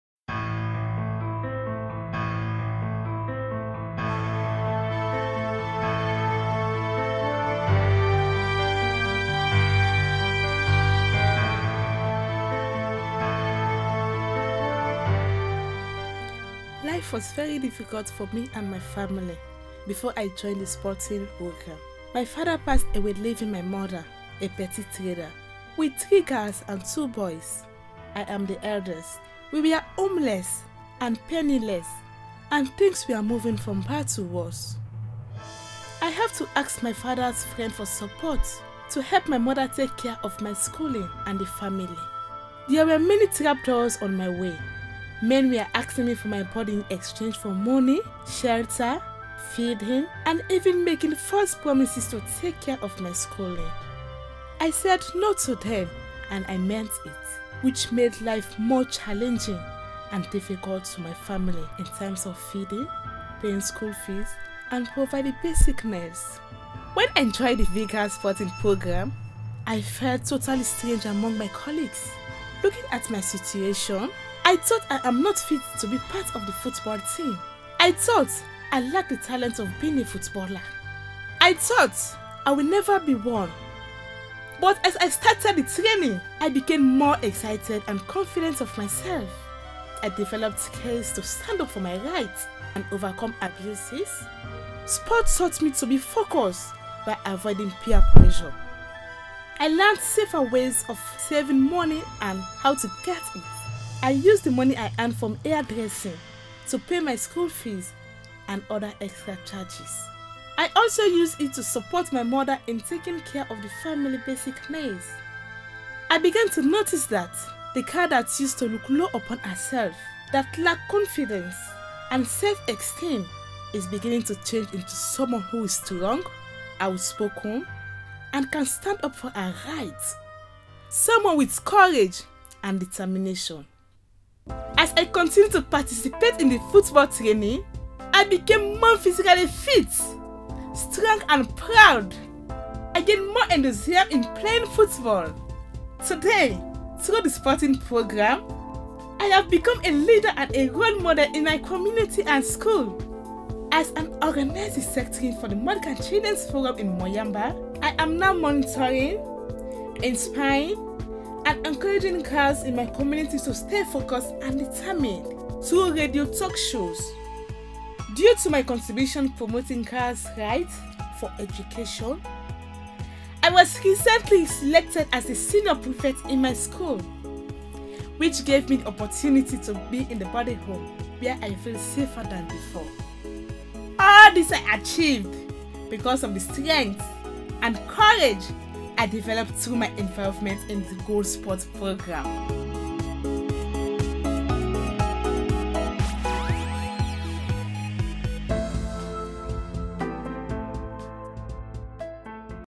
Overcoming Trap Doors
My story is all about how I got myself involved in sport and how it has helped me.